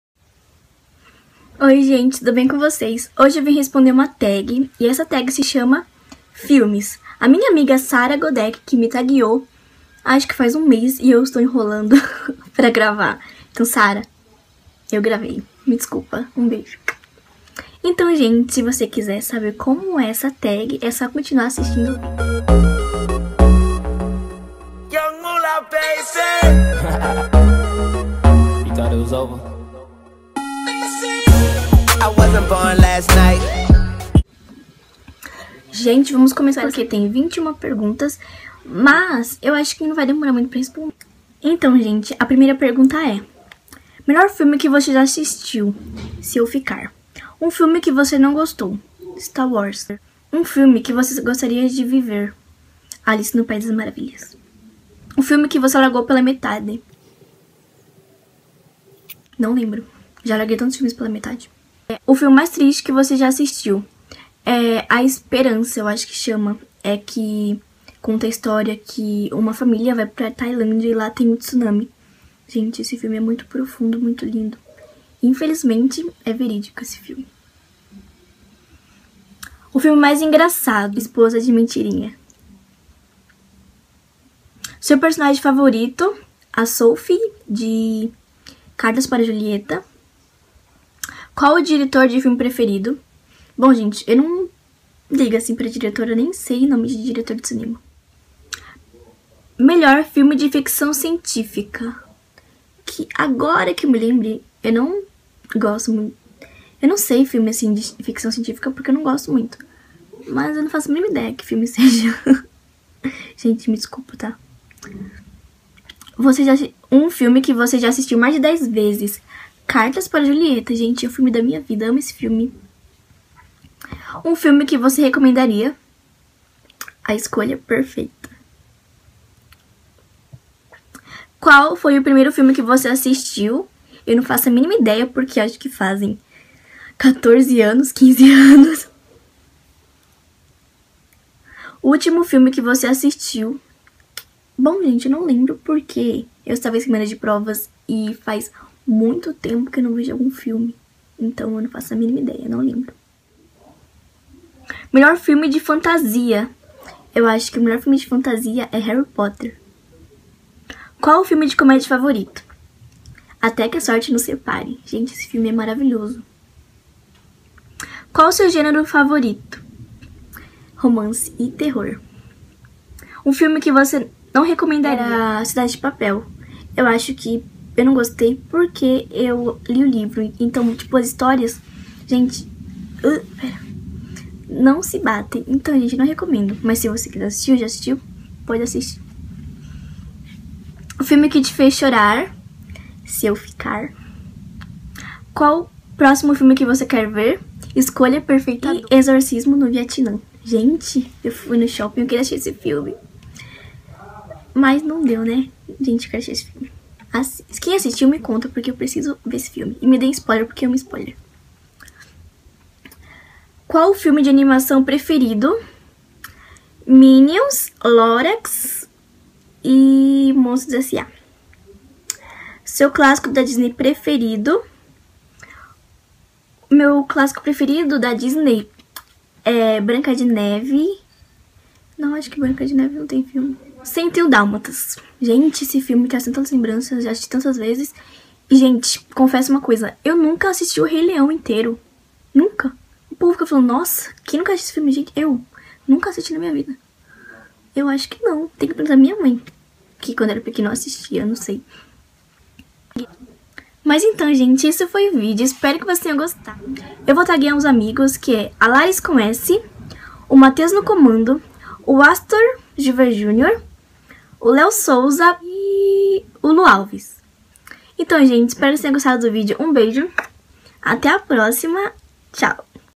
TAG filmes 🎬
Gente, errei heududeu o nome do filme é exorcismo no Vaticano

1- Melhor filme que vc já assistiu
2- um filme que vc não gostou
3- um filme que vc gostaria de viver
4- um filme que vc largou pela metade
5- o filme mais triste que vc já assistiu
6- o filme mais engraçado
7- o seu personagem favorito
8- qual o diretor de filme preferido
9- melhor filme de ficção científica
10- um filme que vc assistiu mais de 10 vezes
11- um filme que vc recomendaria
12- qual foi o primeiro filme que assistiu ?
13-o ultimo filme que vc assistiu
14- melhor filme de fantasia
15- qual o filme de comédia favorito
16- qual o seu gênero favorito
17- um filme que vc não recomendaria
18- um filme que te fez chorar
19- próximo filme que vc quer ver
20 qual o filme de animação preferido
21- seu clássico da disney preferido :3